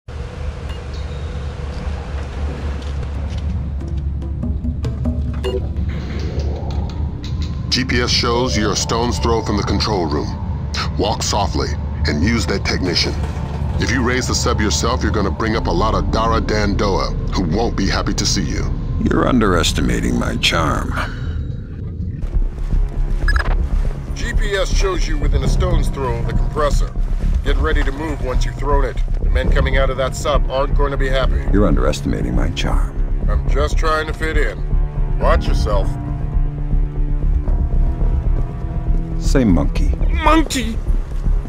Charming Similarity
They didn't bother coming up with new lines, but it's a good opportunity to compare both Lamberts, so which one do you like more, Don Jordan or Dennis Haysbert?